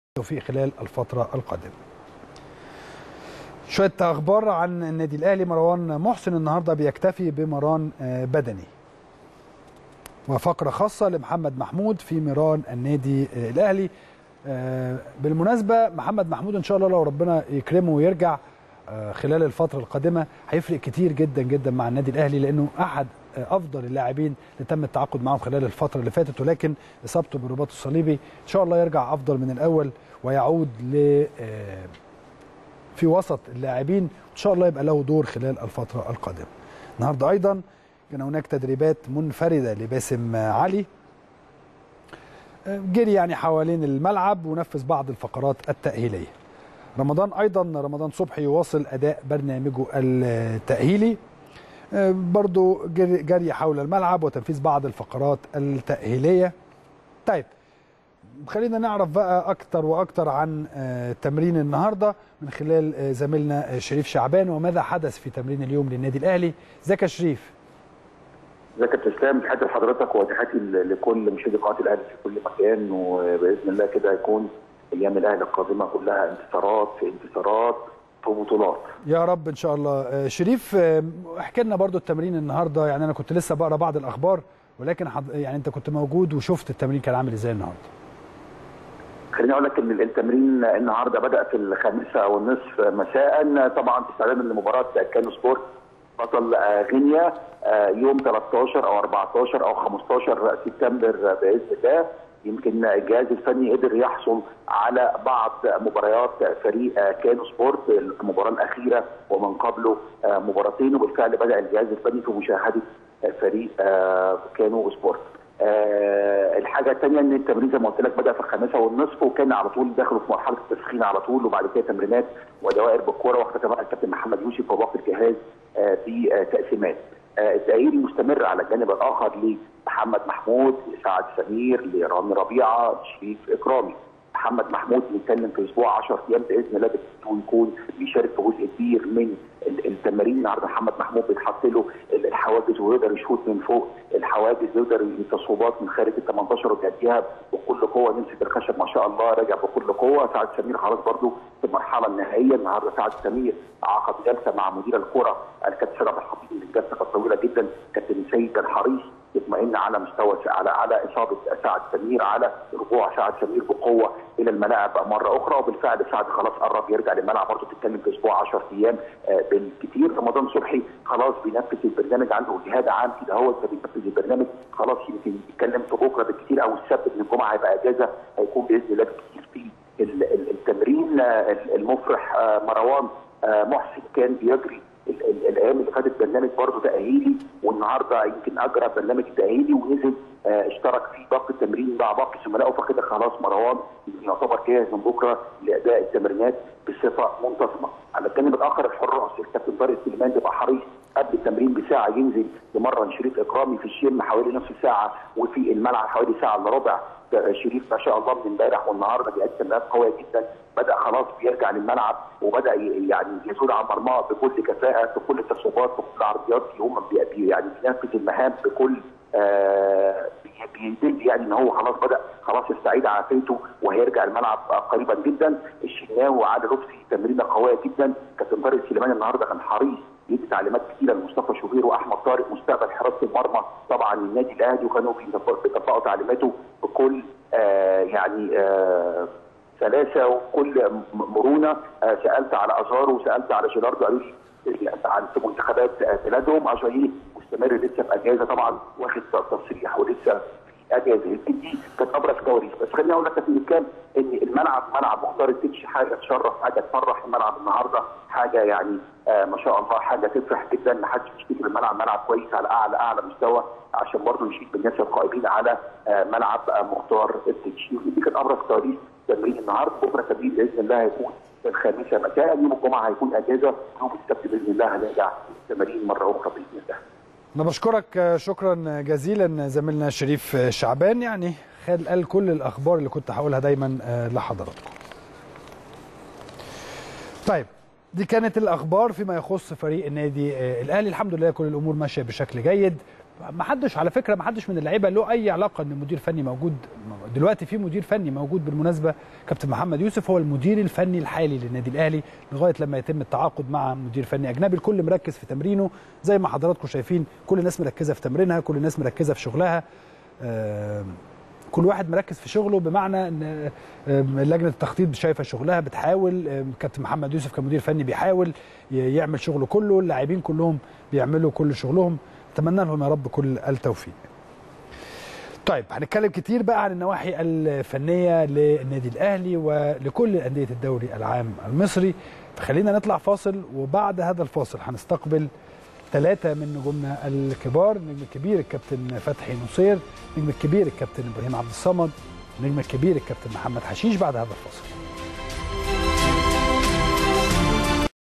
شريف شعبان وكواليس تدريب فريق النادى الاهلى اليوم بملعب التتش
التردد: 12341 أفقي.
القمر: نايل سات.
معامل الخطأ: 5/6.
معدل الترميز: 27500